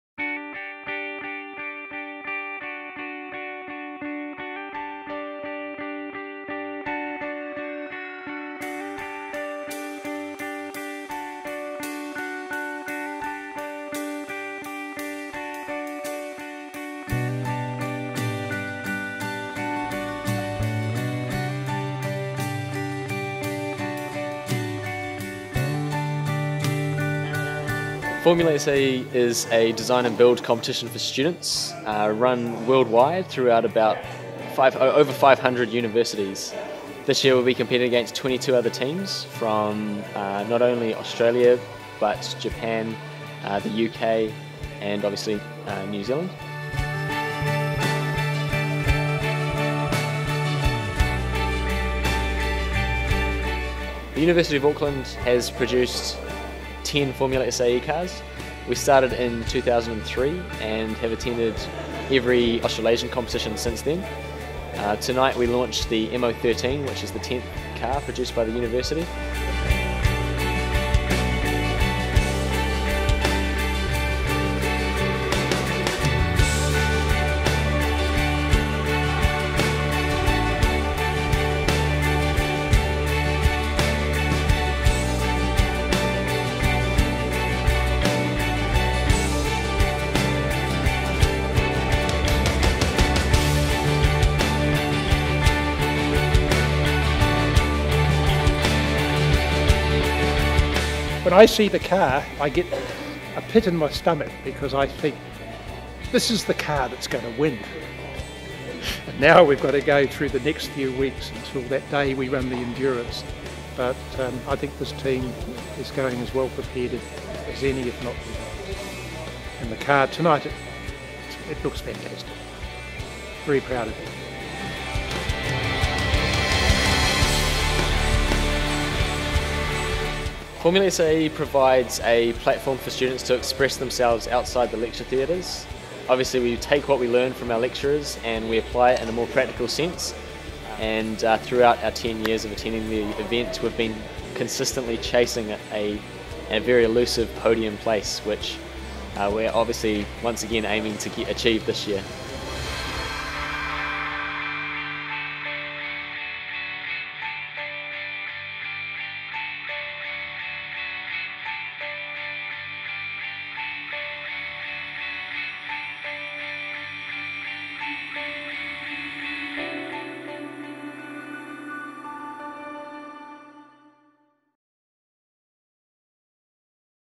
The University of Auckland Formula SAE 2013
The Formula SAE® competition is for SAE student members to conceive, design, fabricate, and compete with small formula-style racing cars. The cars are built with a team effort over a period of about one year and are taken to the annual competition for judging and comparison with vehicles from colleges and universities throughout the world. The University of Auckland FSAE team competes annually in the Australasian FSAE competition. With the support of the University and numerous loyal sponsors, the team strives to not only do them proud, but to cement New Zealand as a leading innovative country. Wish them luck as they prepare for the biggest competitive event of the year: Formula SAE Australasia 2013, 12 December in Melbourne.